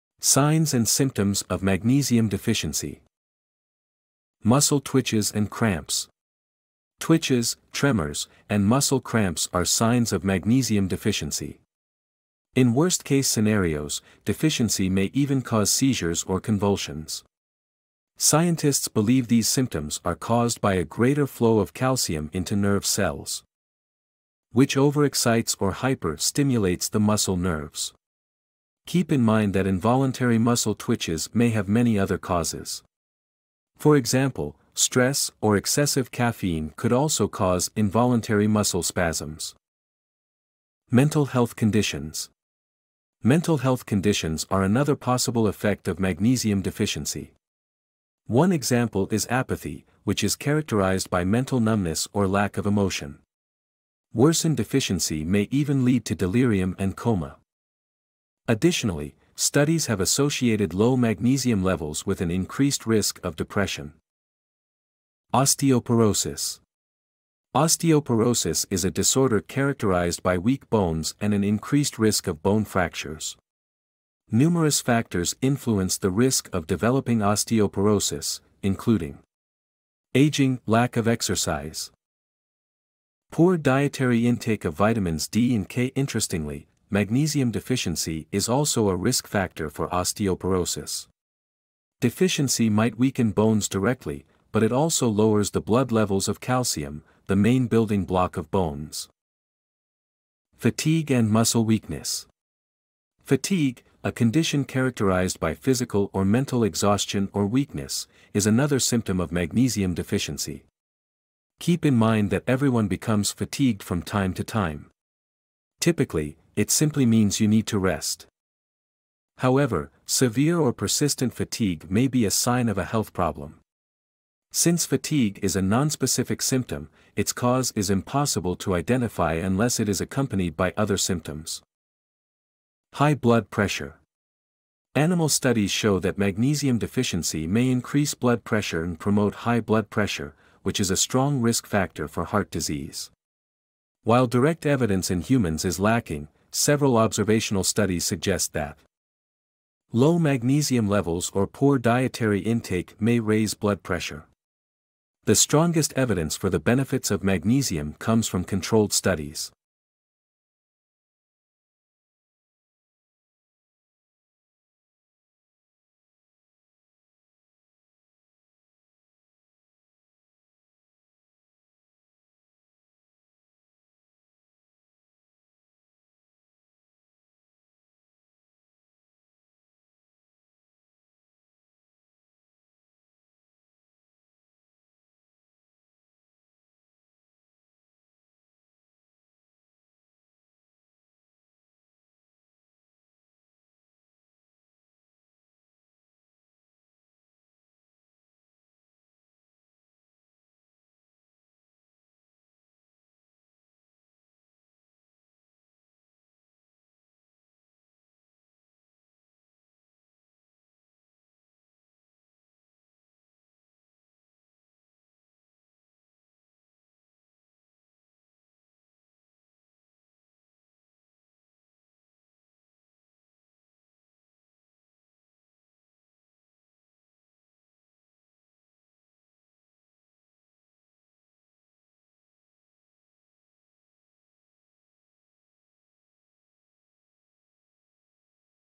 7 SIGNS AND SYMPTOMS OF MAGNESIUM DEFICIENCY || MAGNESIUM DEFICIENCY / viral / trending /short video
7 SIGNS AND SYMPTOMS OF MAGNESIUM DEFICIENCY || MAGNESIUM DEFICIENCY / viral / trending /short video

SIGNS AND SYMPTOMS OF MAGNESIUM DEFICIENCY:
MUSCLE TWITCHES AND CRAMPS
MENTAL HEALTH CONDITIONS
OSTEOPOROSIS
FATIGUE AND MUSCLE WEAKNESS
HIGH BLOOD PRESSURE
ASTHMA
IRREGULAR HEARTBEAT